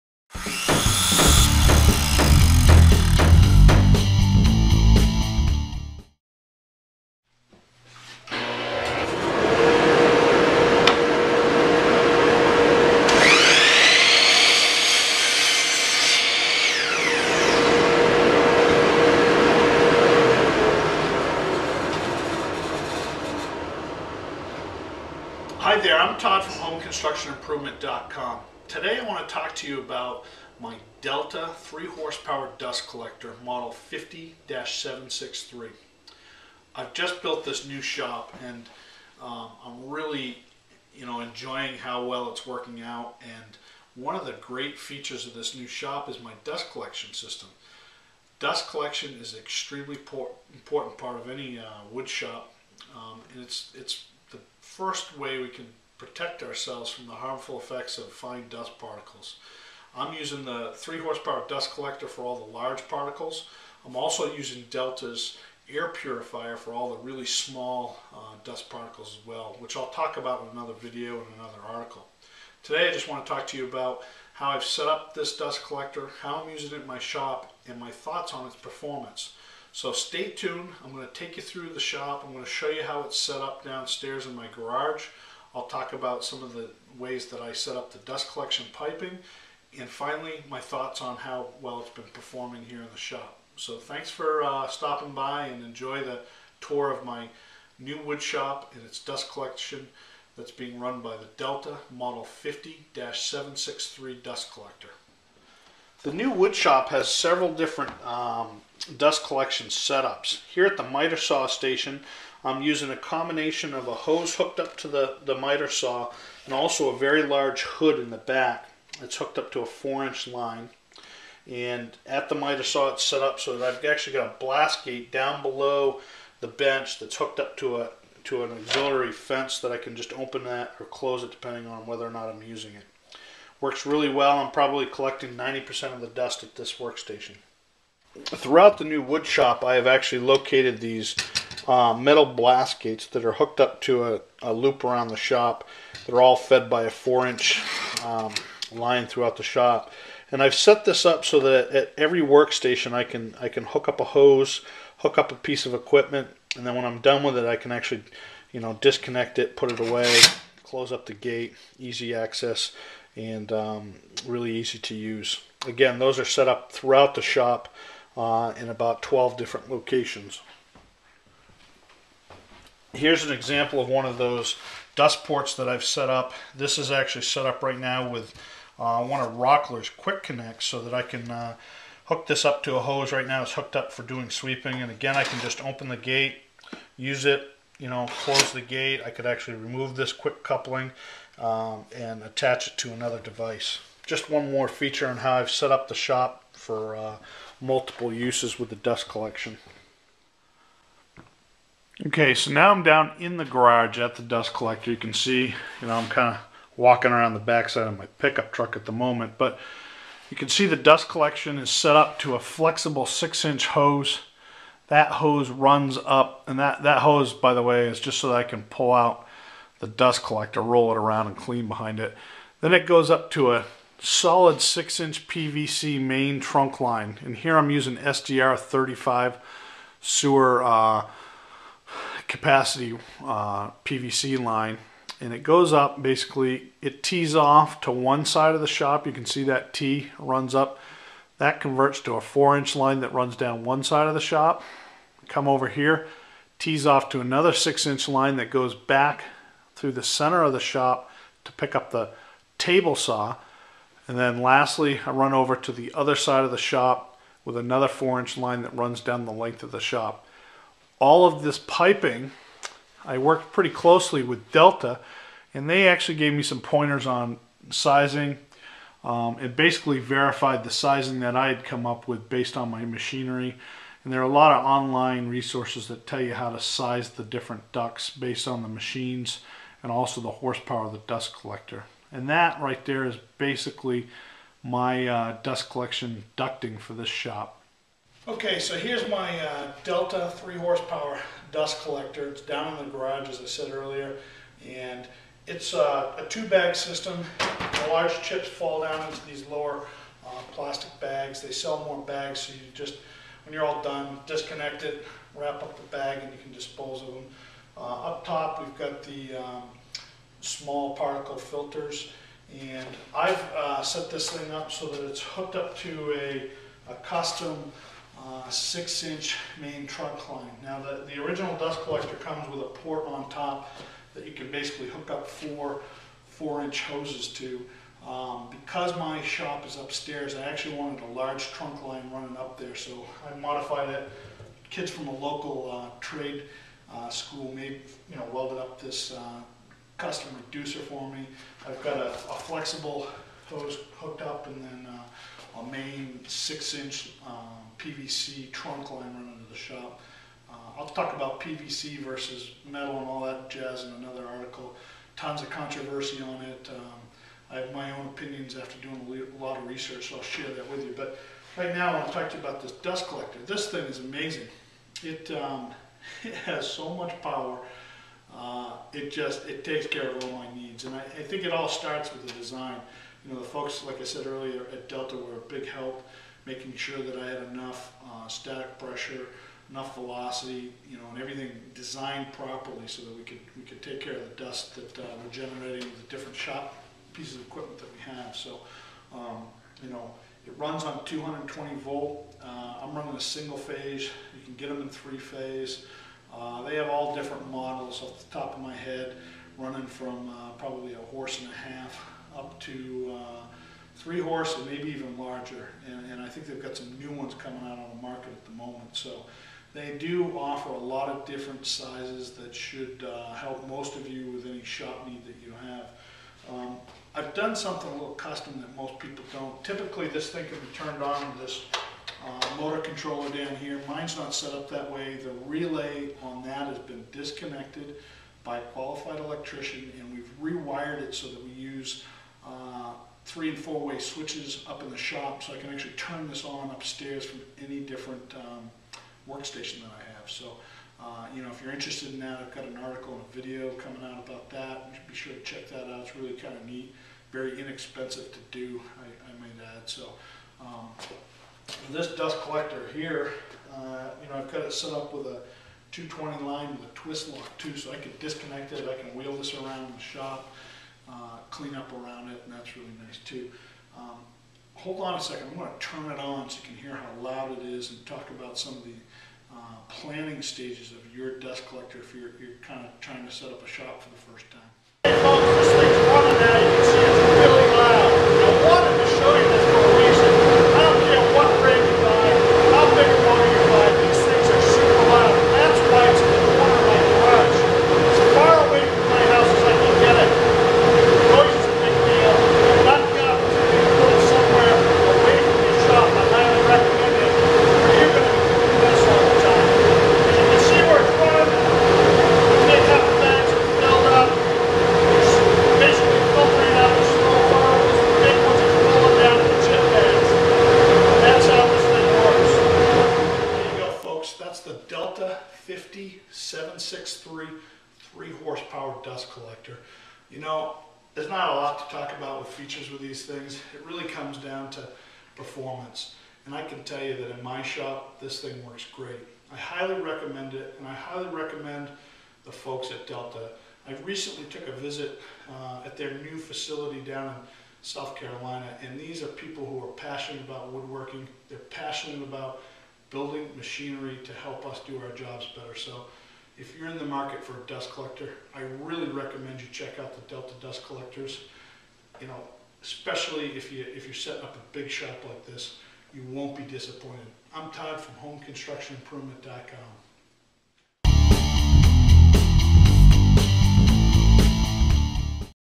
Delta 50-763 Dust Collector Review
talks about his experiences using the Delta Model 50-763 Dust Collector in his new woodshop.